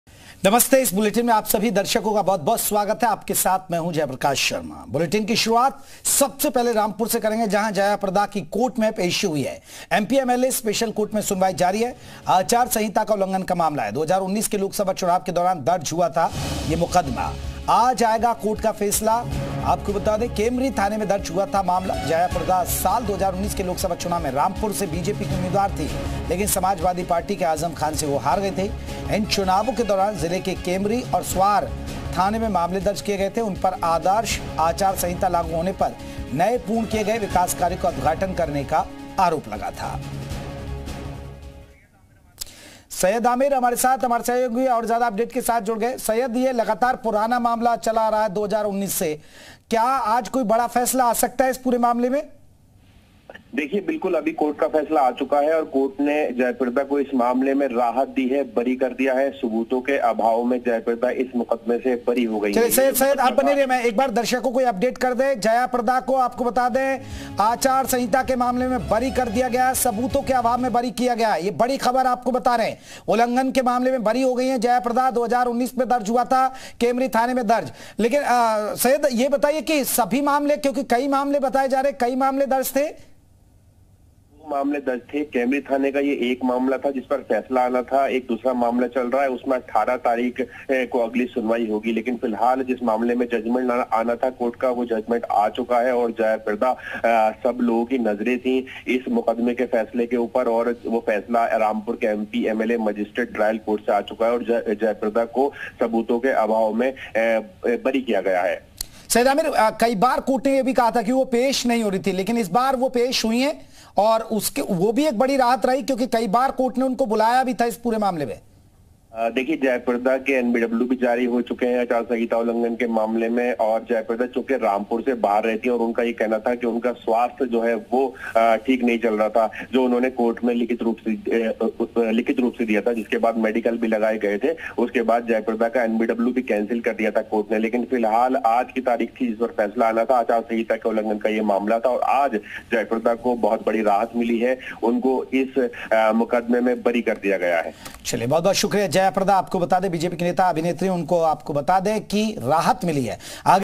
Rampur : जया प्रदा को राहत, आचार संहिता उल्लंघन मामले में हुईं बरी | Jaya Prada | Court
Rampur : जया प्रदा को राहत, आचार संहिता उल्लंघन मामले में हुईं बरी, MP-MLA स्पेशल कोर्ट में हुई सुनवाई, 2019 में दर्ज हुआ था मुकदमा

Rampur: Relief to Jaya Prada, acquitted in code of conduct violation case, hearing held in MP-MLA special court, case was registered in 2019